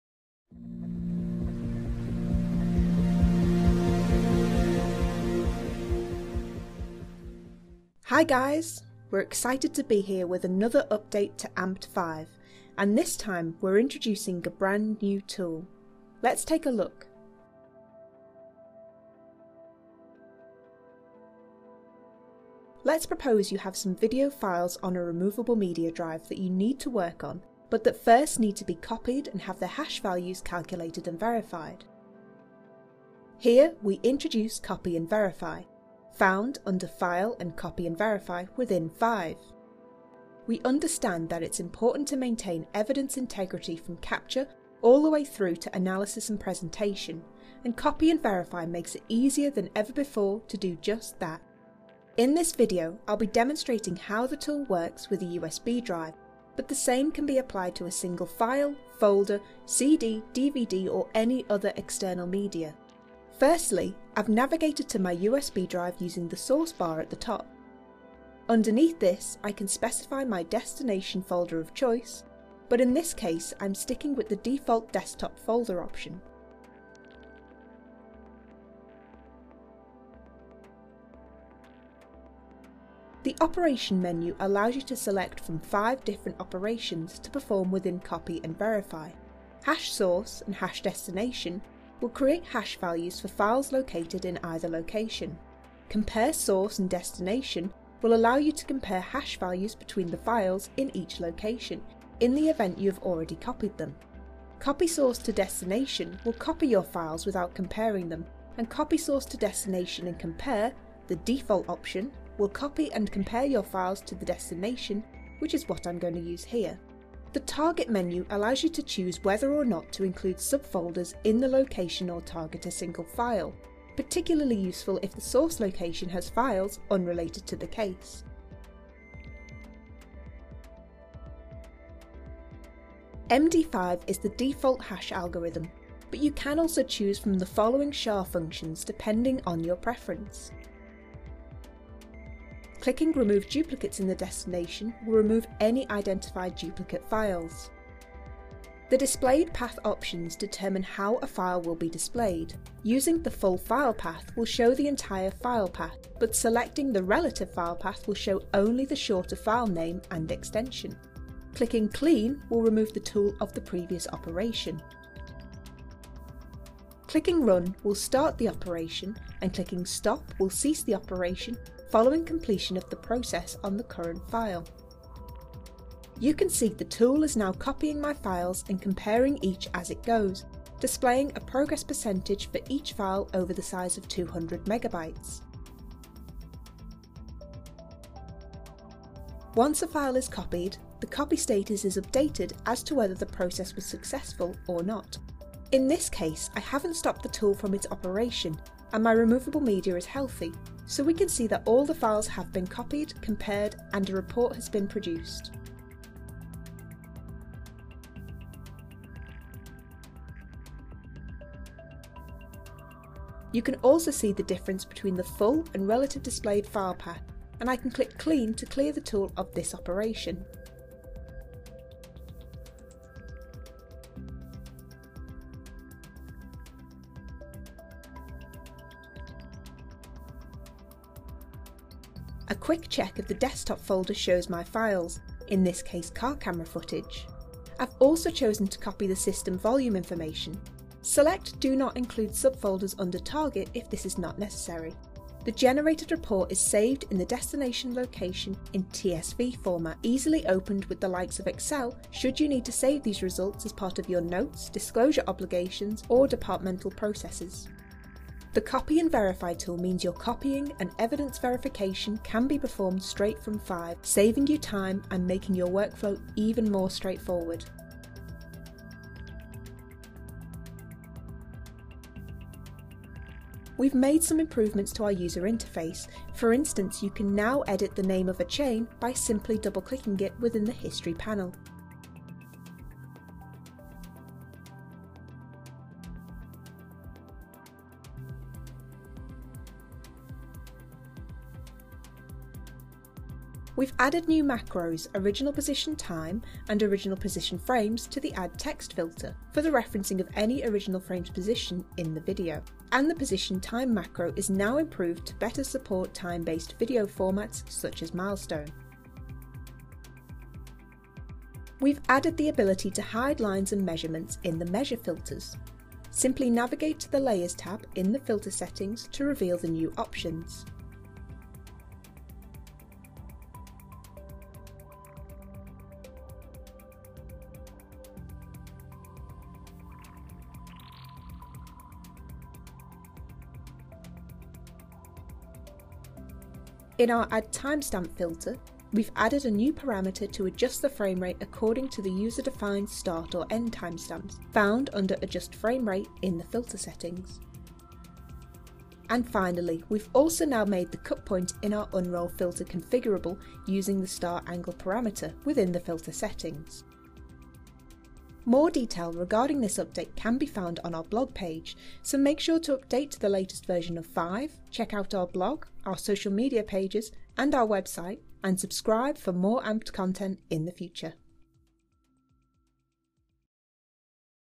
Amped FIVE Update 13609: Copy and Verify Tool and More
Introducing our new Copy and Verify tool and more updated features